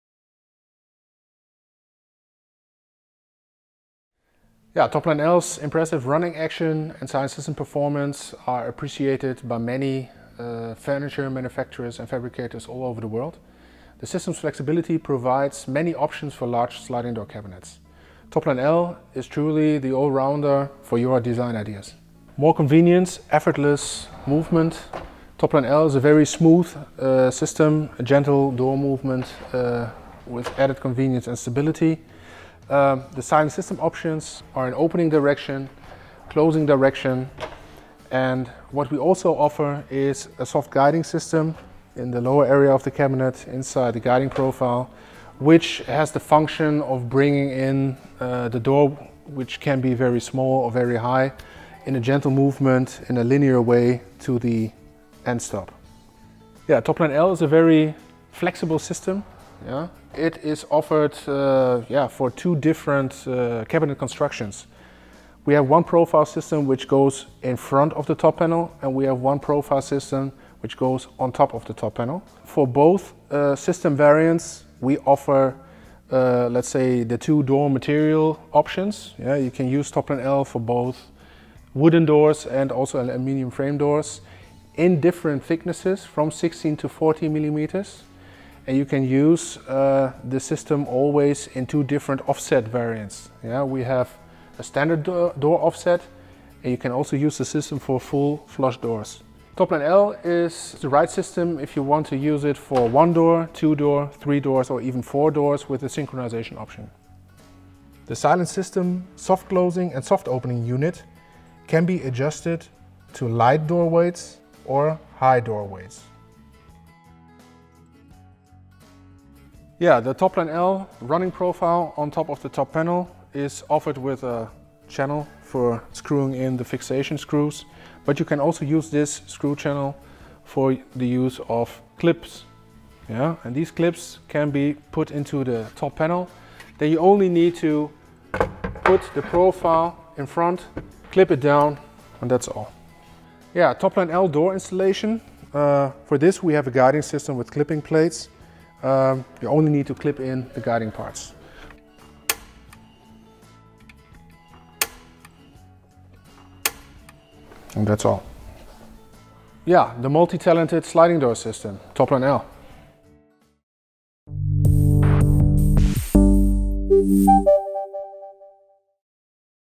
TopLine L sliding door system: technical briefing by Hettich
Versatile sliding door system with perfected Silent System convenience: TopLine L
The sliding door system TopLine L has been developed for the demands of modern living: it is particularly adaptable and impresses with great performance – perfect for individualized design solutions.

The following videos are also worth seeing. Have a look!